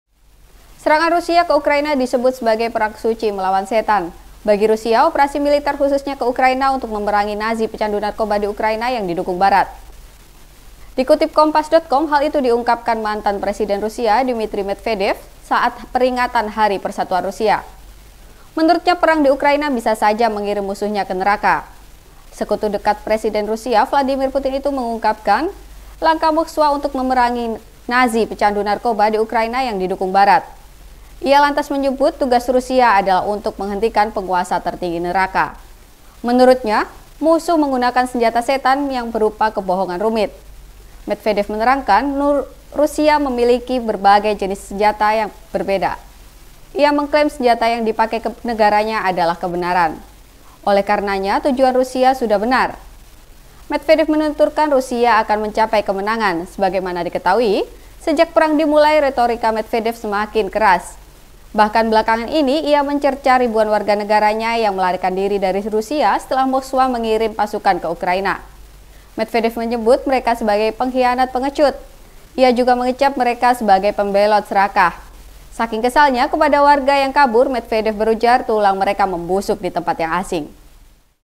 Rusia Sebut Serangan ke Ukraina Jadi 'Perang Suci Lawan Setan', Basmi 'Nazi' yang Didukung Barat
SURYA.CO.ID - Serangan Rusia ke Ukraina disebut sebagai perang suci melawan setan.

Bagi Rusia, operasi militer khususnya ke Ukraina untuk memerangi “Nazi pencandu narkoba” di Ukraina yang didukung Barat.

Dikutip Kompas.com, hal itu diungkapkan Mantan Presiden Rusia Dmitry Medvedev saat peringatan Hari Persatuan Rusia.

Menurutnya, perang di Ukraina bisa saja mengirim musuhnya ke neraka.

Sekutu dekat Presiden Rusia Vladimir Putin itu mengungkapkan, langkah Moskwa untuk memerangi “Nazi pencandu narkoba” di Ukraina yang didukung Barat.

Ia lantas menyebut tugas Rusia adalah untuk menghentikan “penguasa tertinggi neraka”.

 Menurutnya, musuh menggunakan senjata setan yang berupa kebohongan rumit.

Medvedev menerangkan, Rusia memiliki berbagai jenis senjata yang berbeda.Ia mengklaim senjata yang dipakai negaranya adalah kebenaran.

Oleh karenanya tujuan Rusia sudah benar.

Medvedev menuturkan, Rusia akan mencapai kemenangan.

Sebagaimana diketahui, sejak perang dimulai, retorika Medvedev menjadi semakin keras.Bahkan belakangan ini, ia mencerca ribuan warga negaranya yang melarikan diri dari Rusia setelah Moskwa mengirim pasukan ke Ukraina.

Medvedev menyebut mereka sebagai "pengkhianat pengecut".

Ia juga mengecap mereka sebagai pembelot serakah.

Saking kesalnya kepada warga yang kabur, Medvedev berujar tulang mereka membusuk di tempat asing.